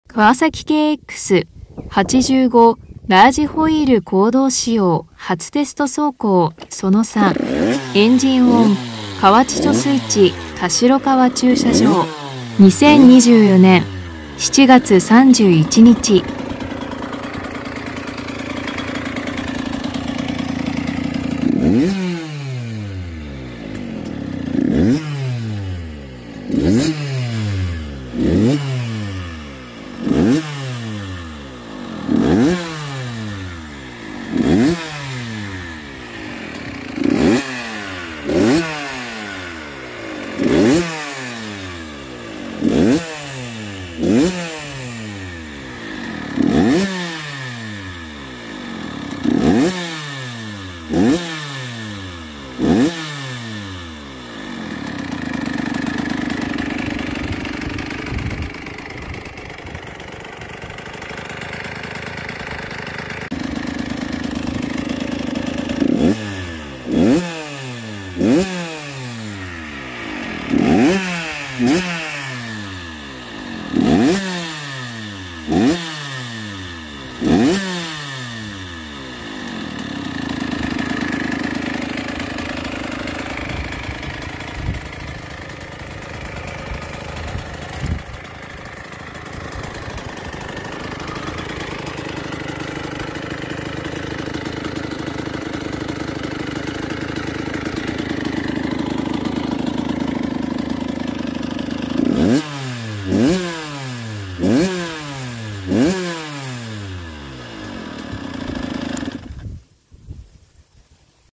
240731＃3カワサキKX85ⅱ初テスト走行 エンジン・排気 田代川駐車場 河内貯水池
エンジン音及び排気の様子です。
2ストオイルも初めてのモノを使用。
1996年にモトクロスを引退する時にはカストロールA747を使ってましたが終売。
大昔はシェルスーパーM、カストロールR30とかも。
今回はエルフMOTO2のオフロード用です。
混合比は40対1にしてます。